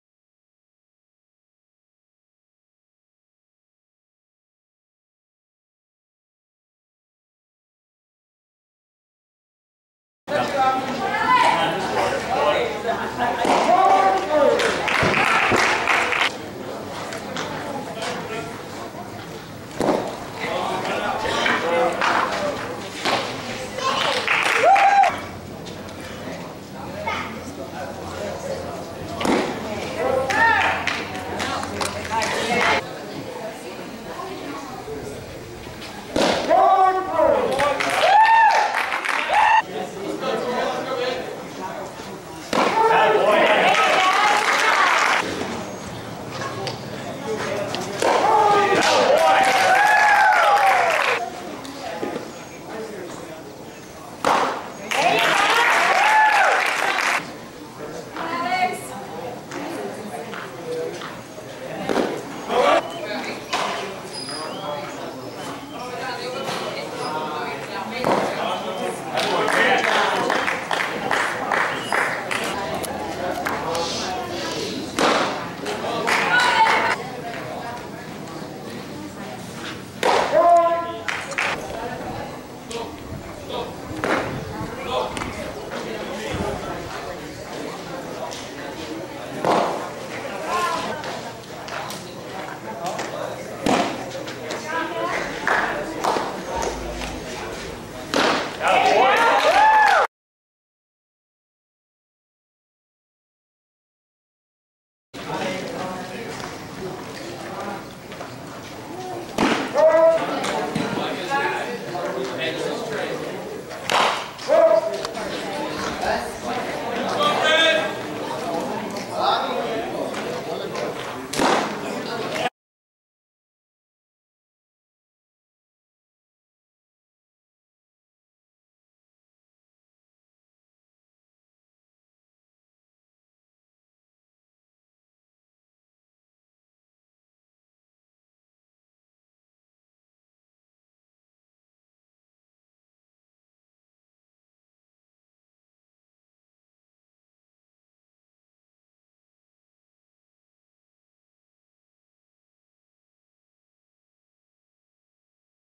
Chance Riley vs Score International 17U, July 2014
7 Innings pitched, 9 Ks, 2 BB, 1 ER, and 4 hits.